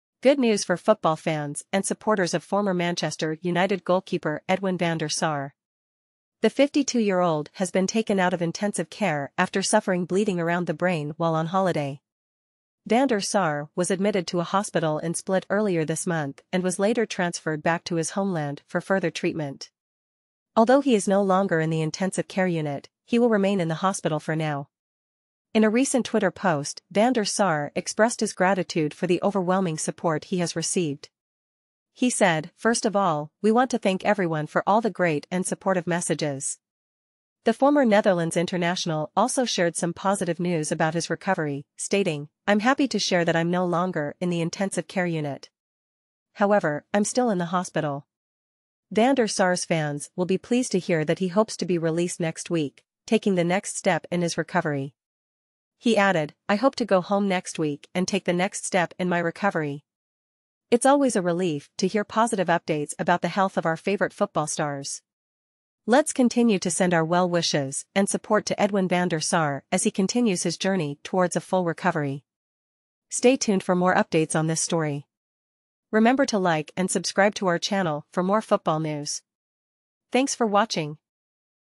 Former Manchester United goalkeeper Edwin van der Sar out of intensive care
The former Netherlands and Manchester United goalkeeper Edwin van der Sar has been taken out of intensive care.

The 52-year-old was taken into hospital in Split this month after suffering bleeding around the brain whilst on holiday before he moved to a hospital back in his homeland.

Although Van der Sar has been taken out of the intensive care unit, he will remain in hospital but hopes to be released next week.

In a post on Twitter, he said: “First of all, we want to thank everyone for all the great and supportive messages.

“I’m happy to share that I’m no longer in the intensive care unit. However, I’m still in hospital. I hope to go home next week and take the next step in my recovery!”